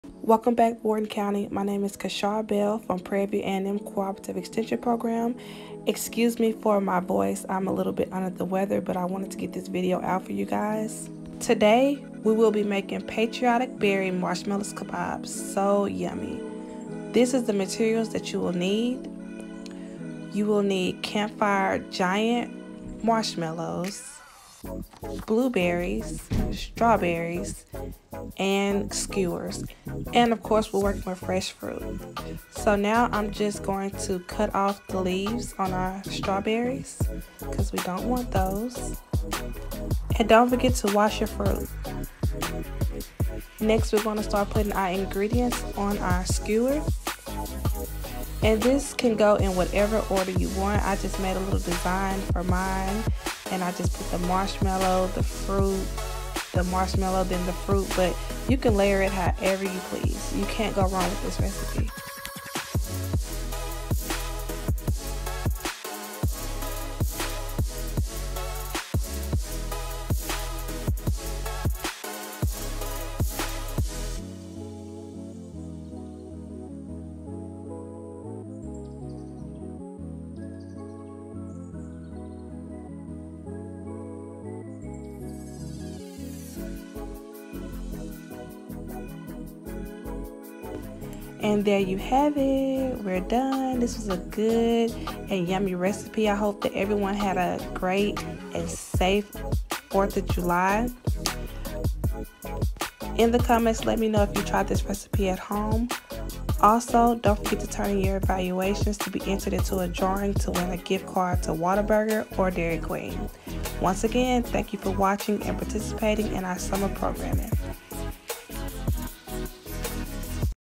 PVAMU CEP-Patriotic Marshmallow Kabobs
Welcome back Wharton County! Please excuse my voice during this recording. Our recipe today is Patriotic Berry Marshmallow Kabobs! Which are always popular during summer celebrations. Please watch the entire video for a specials surprise! Don’t forget to pick up your follow along bags starting on Monday-Wednesday at your local library. If you have any questions,  please contact your local library in Wharton County Texas. I hope each of you continue to enjoy our summer programming! See you on our next video!